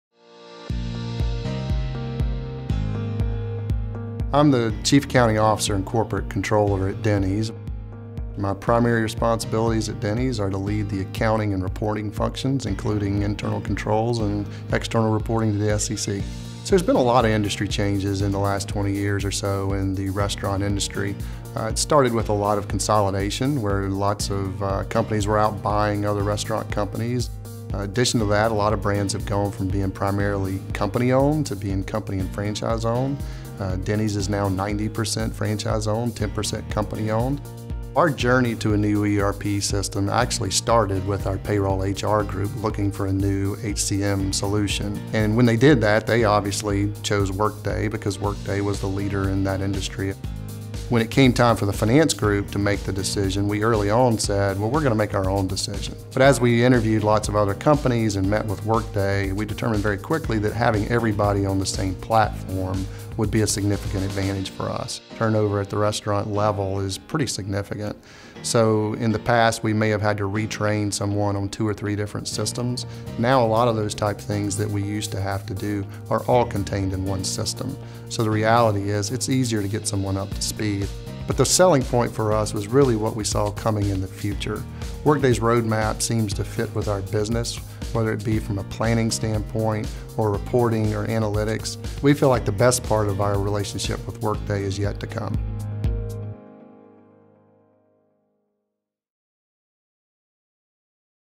Workday and Denny’s
Hear Denny’s Chief Accounting Officer, Jay Gilmore, share the value Denny’s saw in having all of its employees on a single platform for finance and HR with Workday.